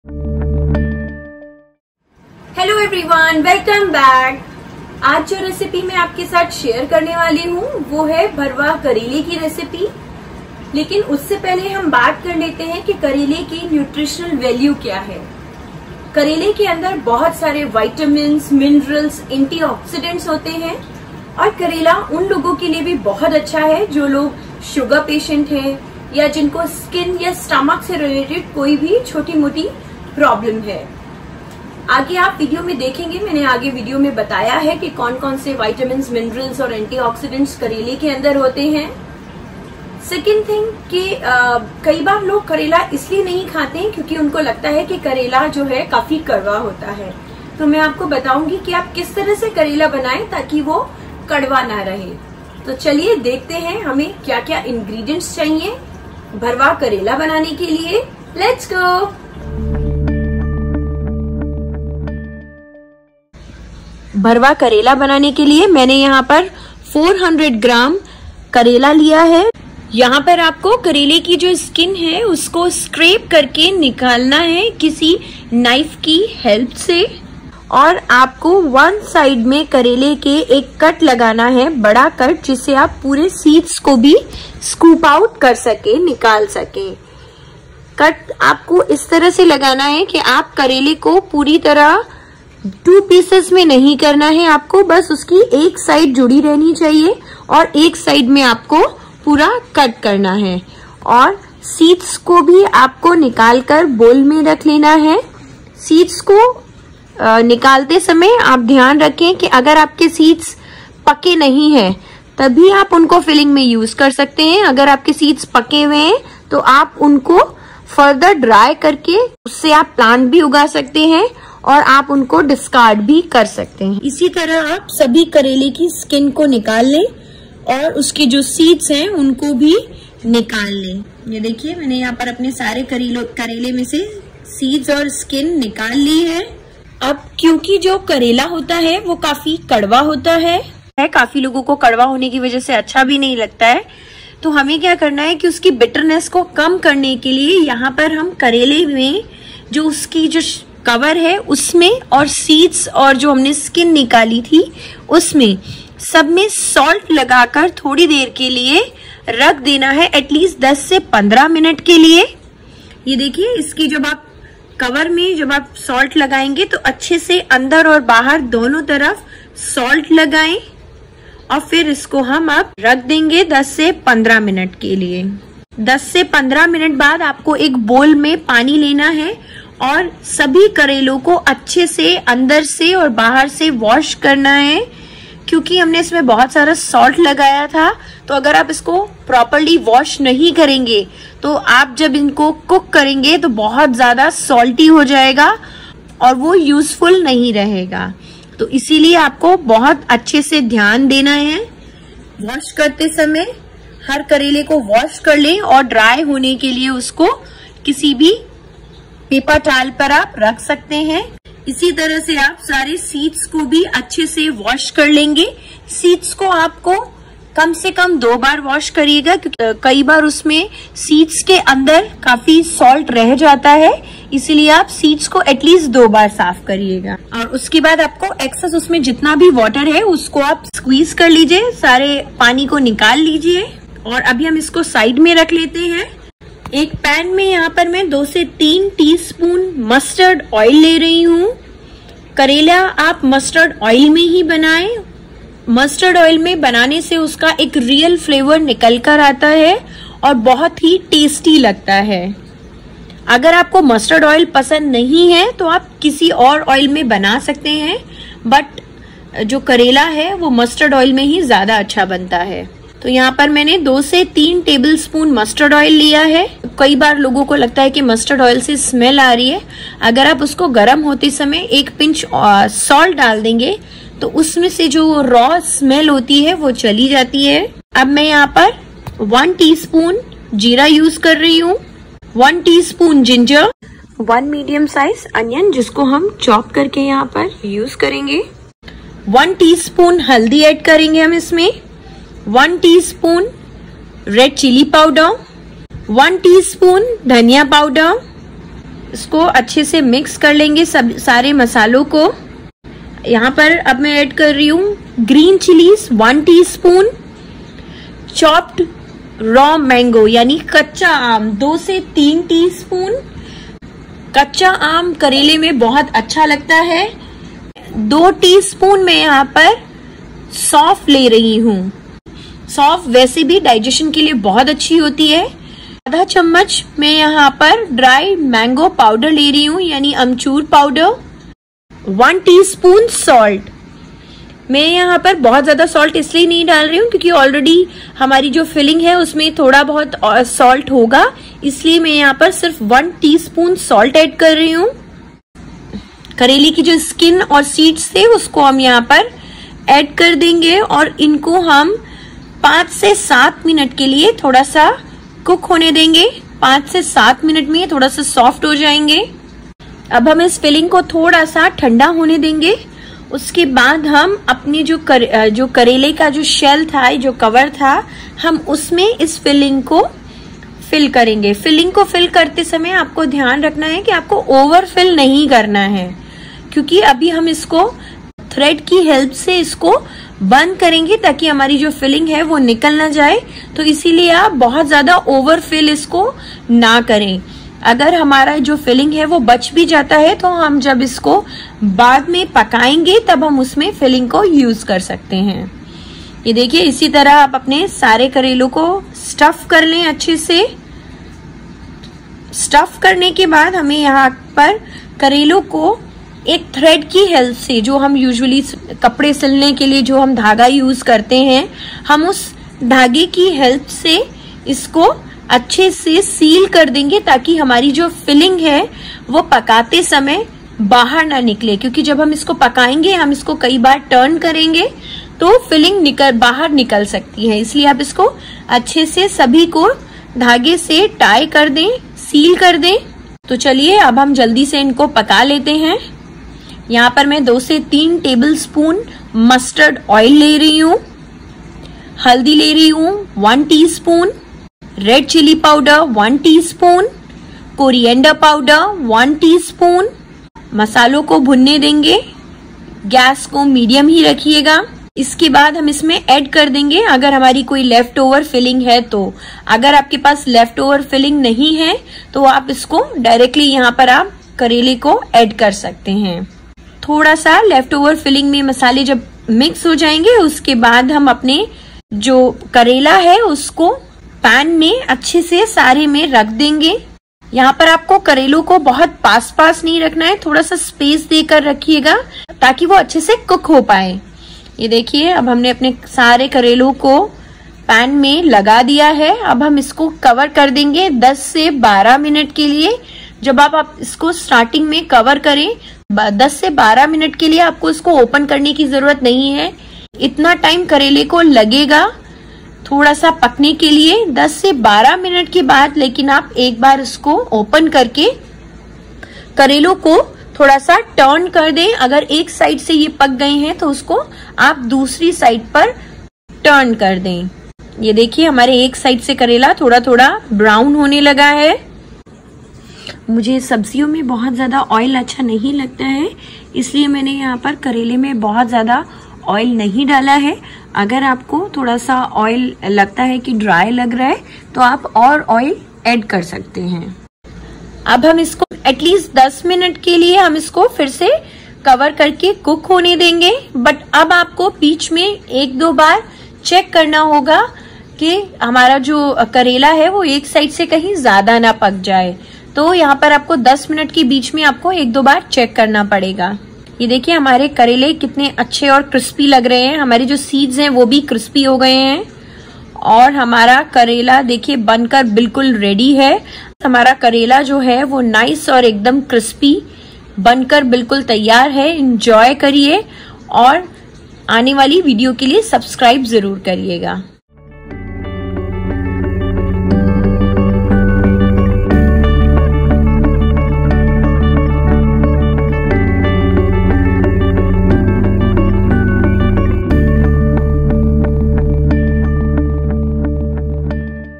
Bharwa Karela | भरवां करेला | How to make Stuffed Karela Easily | Stuffed Bitter Melon/Gourd
Bharwa Karela/Bitter Gourd

Ingredients
Bitter Gourd/Karela 400g
Green mango 2tsp
Green chilli 1tsp
Ginger 1tsp
Cumin/Jeera 1tsp
Haldi 2tsp
Dry mango powder 1tsp
Salt 1tsp
Mustard oil 4-5tbsp

Instructions
Scrape all the karela, slit from one side and scoop out the seeds. Keep the skin and seed and add salt in it and the shell of the karela.
Let this sit for 10-15 min. After that wash them thoroughly, squeeze out the excess water from the seeds and skin.
In a pan add mustard oil, cumin, haldi, red chilli powder and coriander powder and Chopped onion and cook till golden brown.
Then add filling, raw mango, fennel seeds and salt. Let it cook for 5-7 min.
Now stuff the karela with the prepared filling (don't overfill)and seal them with thread.
In a pan add oil, haldi, red chilli powder and coriander powder, let the spies roast. Then add the left over filling  and cook for another 2-3 min.
Add the stuffed karela, cover and cook for 10 min. After 10 min check and turn  the karela if one side is golden brown.
Let this cook for another 10-12 min, this time you have check the karela in between to avoid burning.
When cooked serve hot with the meal.